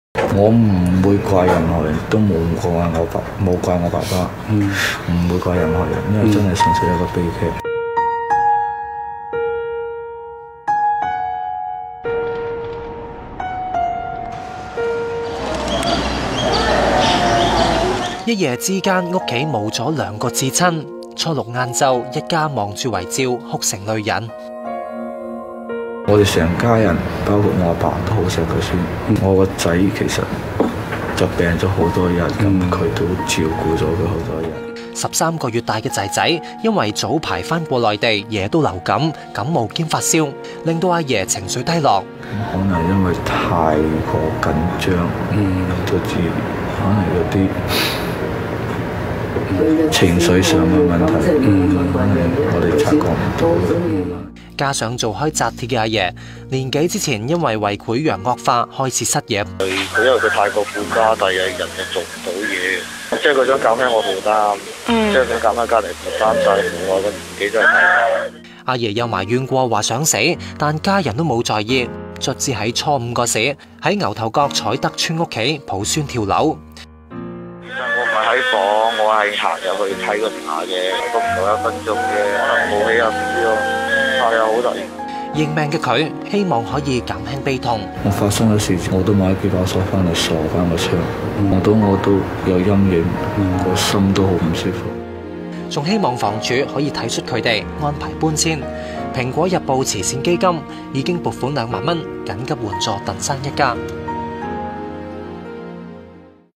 爺孫墮樓 B爸：唔怪我爸爸
【本報訊】發生爺爺抱孫跳樓同死悲劇的牛頭角彩德邨單位，家人今日在窗花上加扣上兩把鎖，令現場氣氛倍添哀愁。一夜間喪父亡兒的苦命人鄧先生，以一句「認命」來減輕自己悲痛，指事發時他剛入房取手機，不料父親就抱着孫兒從廳窗躍下慘死，他聲稱不會怪責父親，可能父親太過顧家致壓力爆煲，抑鬱成疾做出此事。